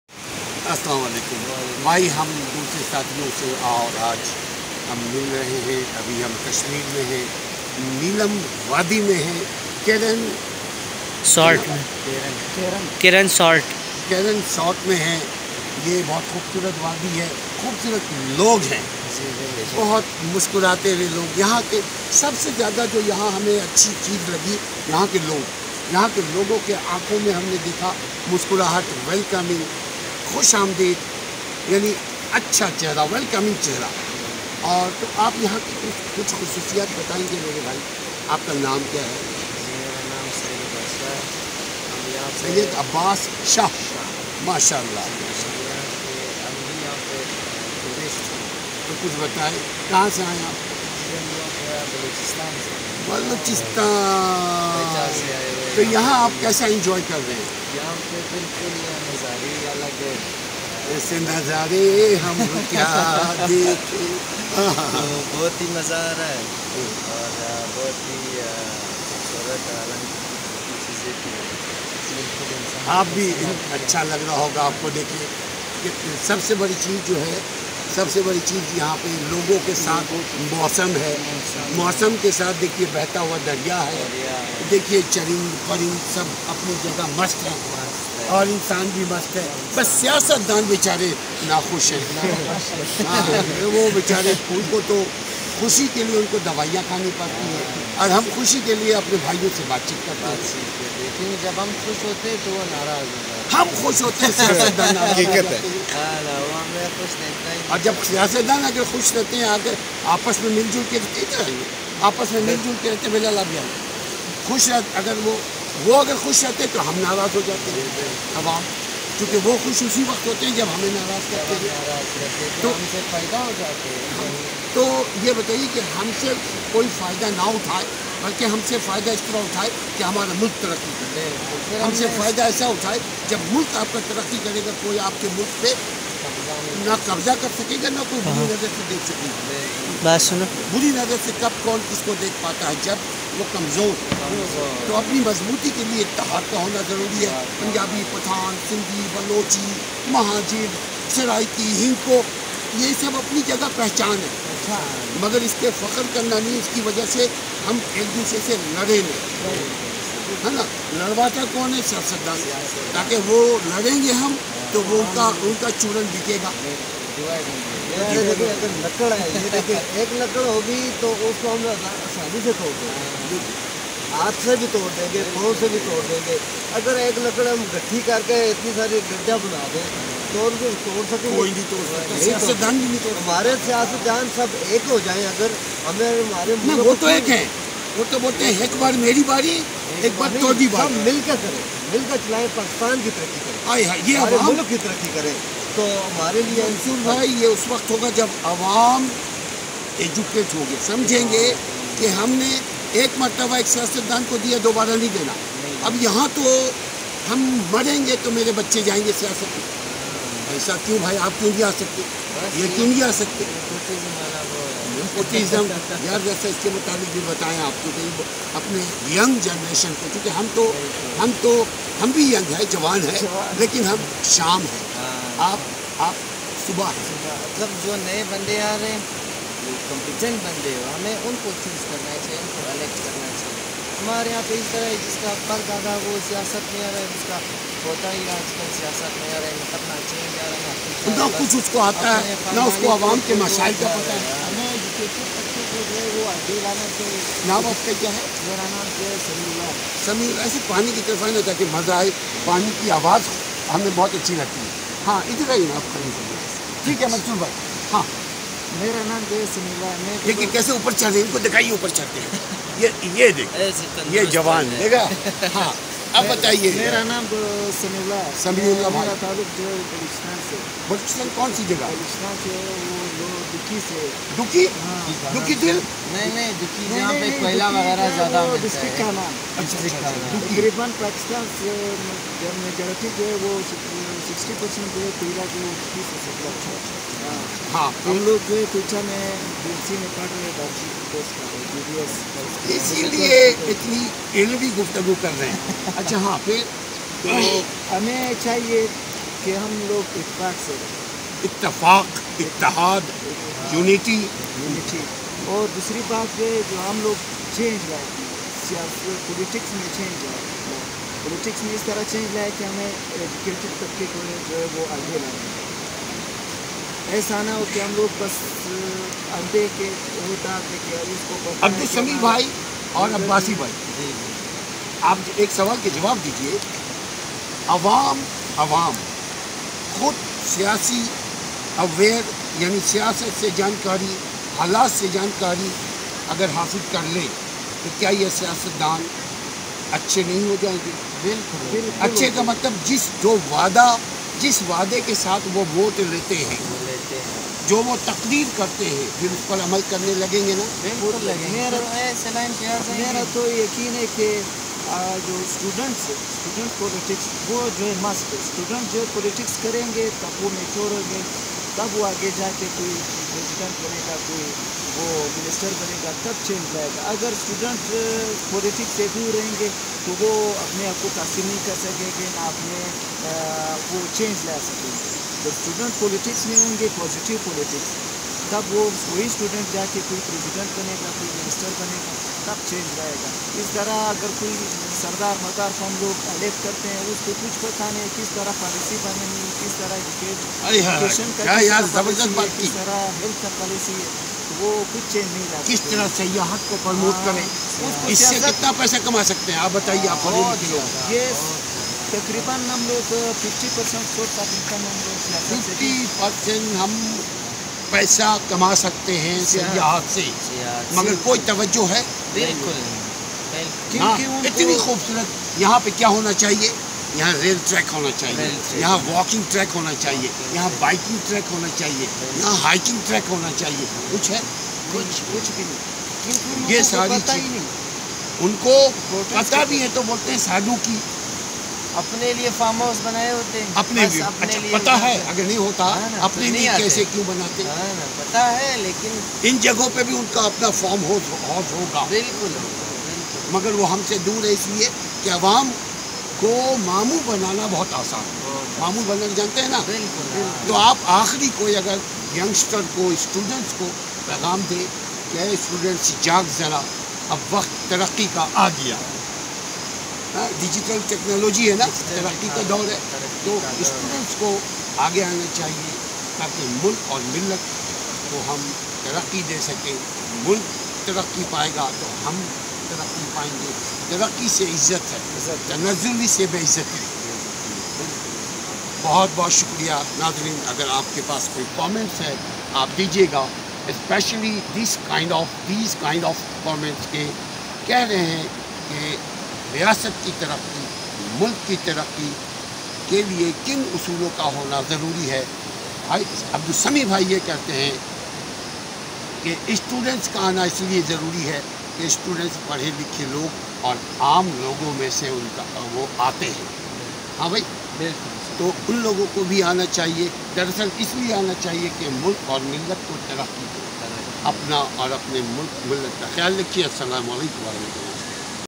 Travelling to Keran Village Neelum Valley Kashmir دنیا کی جنت کشمیر
Travelling to Keran Village Neelum Valley Beautiful Kashmir Ki Wadi  A Paradise on Earth Talking with S Abbas Shah